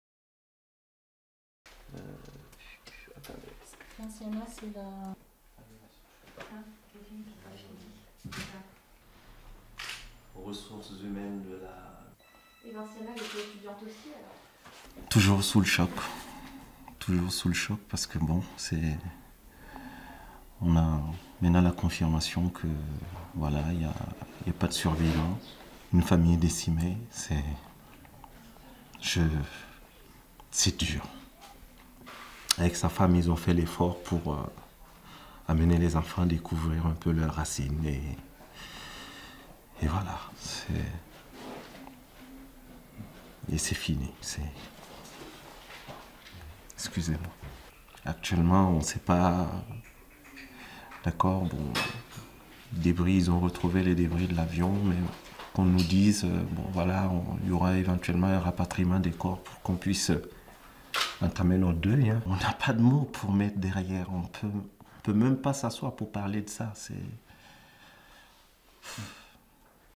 Crash d'Air Algérie: le frère d'une victime témoigne
Témoignage de M. Amadou Ouedraogo, qui a perdu son frère, sa belle-soeur et leurs quatre enfants, ainsi qu'un neveu, dans le crash de l'avion d'Air Algérie, qui s'est écrasé jeudi 24 juillet dans le nord du Mali. Durée 01:10